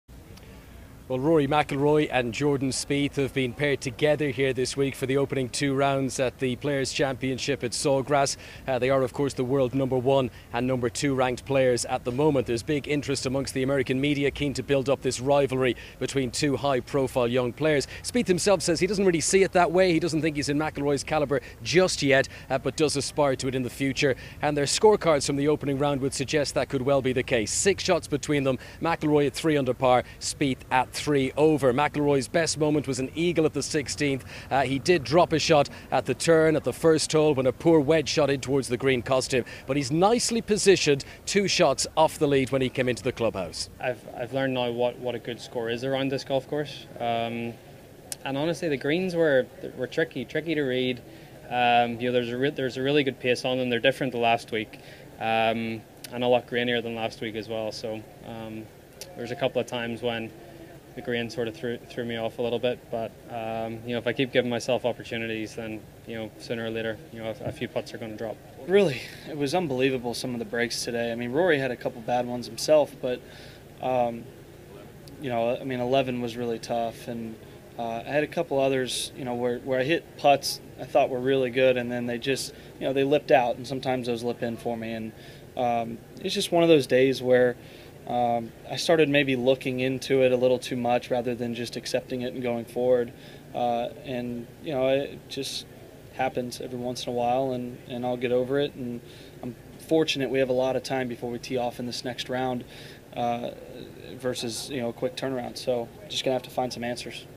Round One Report TPC Sawgrass 2015
Rory McIlroy and Jordan Spieth experience contrasting opening rounds